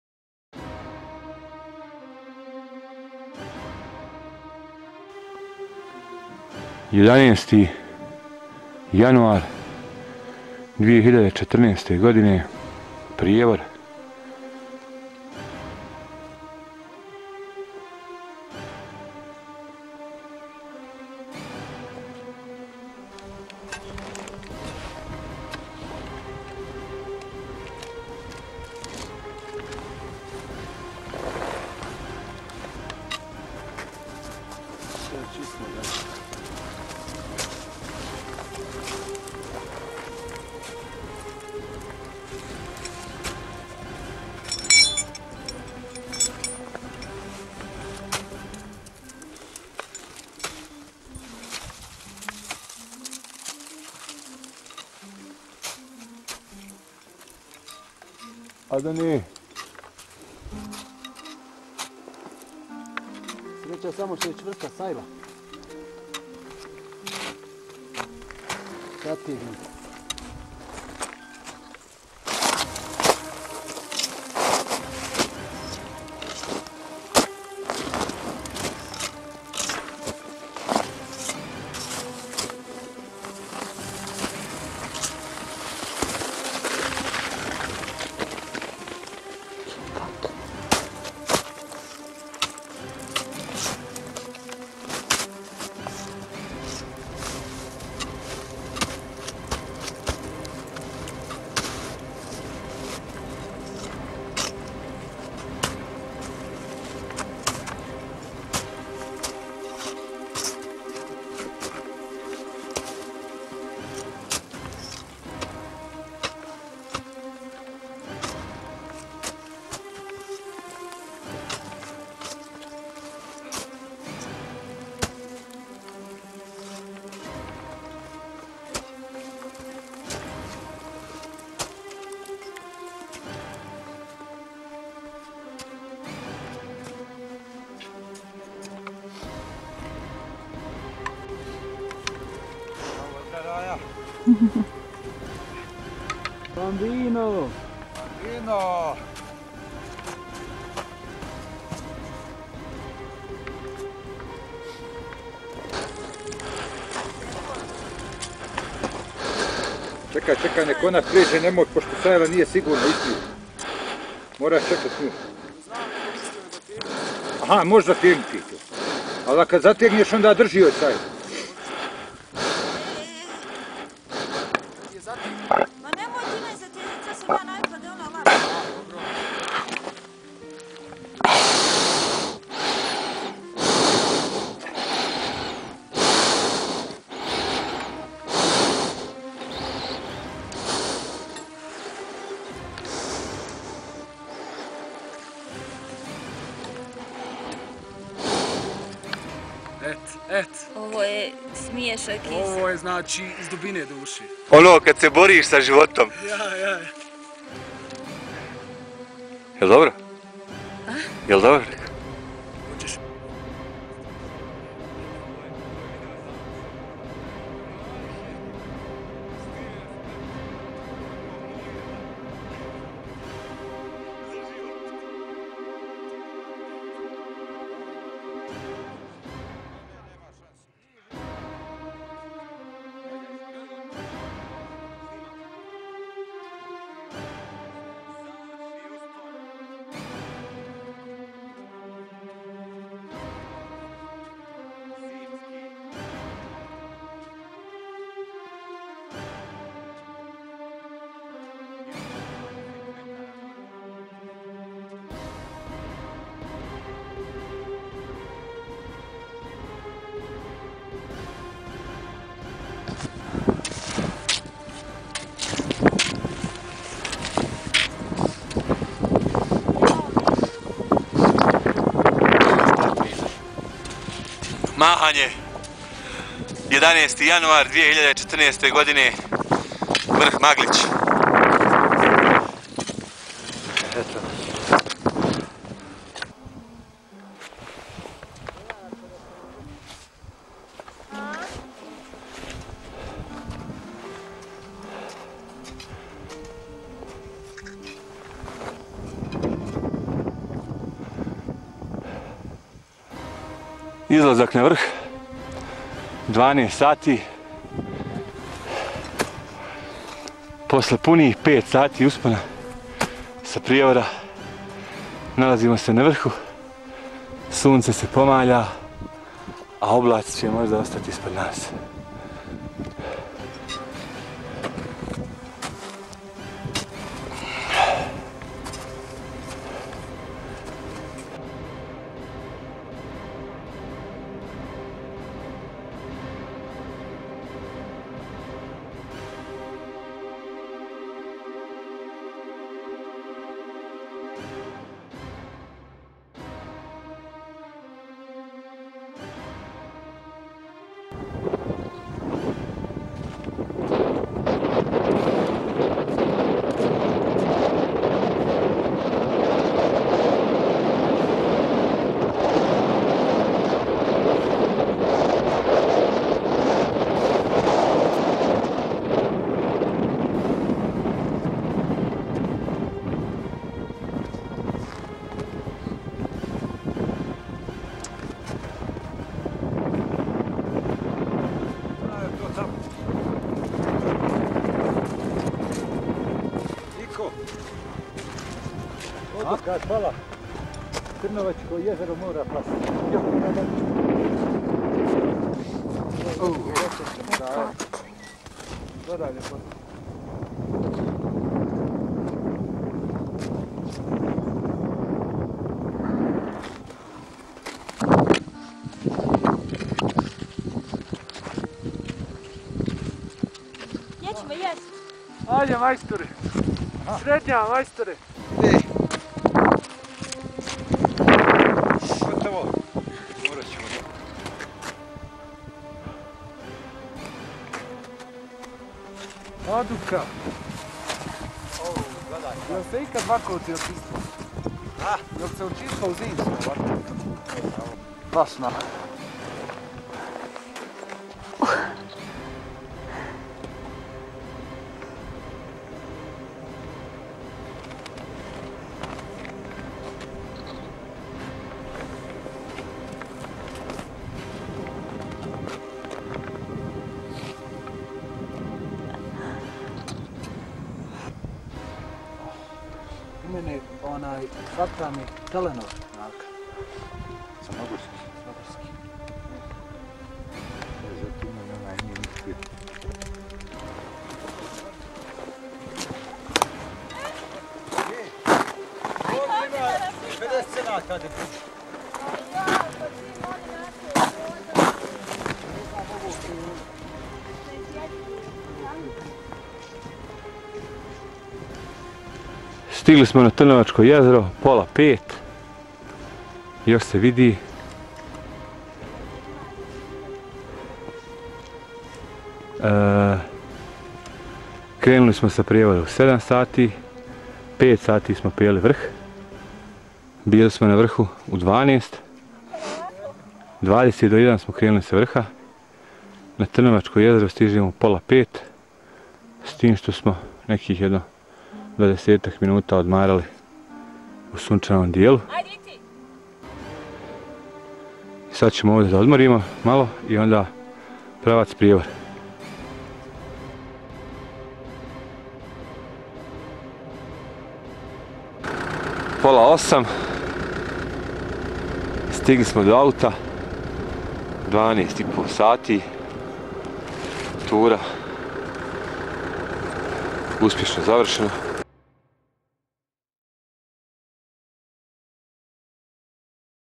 ZIMSKI USPON NA MAGLIĆ 11.01.2014.
USPON NA MAGLIĆ SA PRIJEVORA SILAZAK NA TRNOVAČKO JEZERO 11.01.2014.